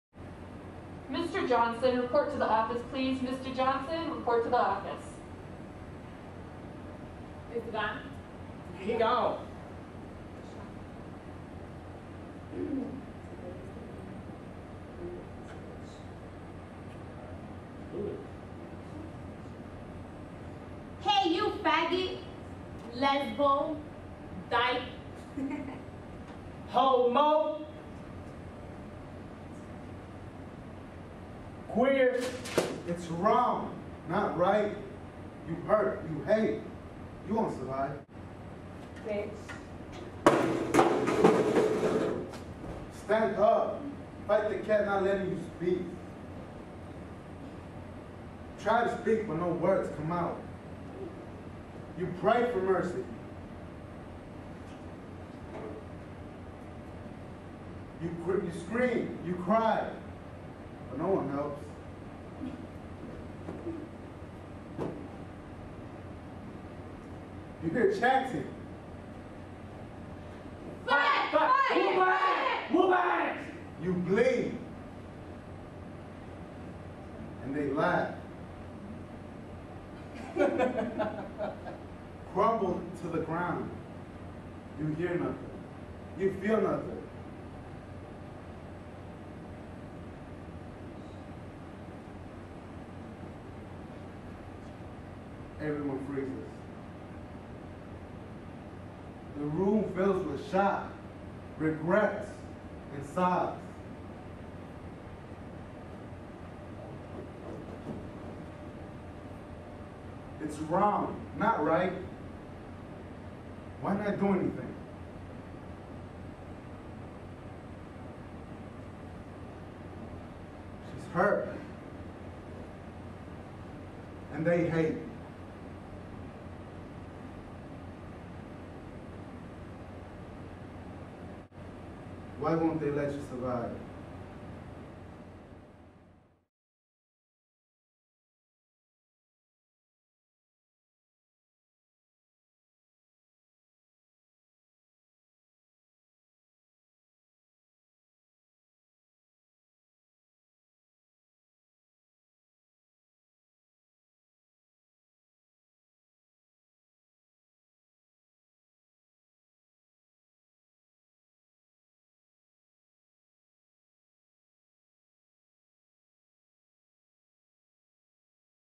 ECA High School: A Needle in a Haystack
Original theater performance by  El Cuarto Año High School students: A Needle in a Haystack.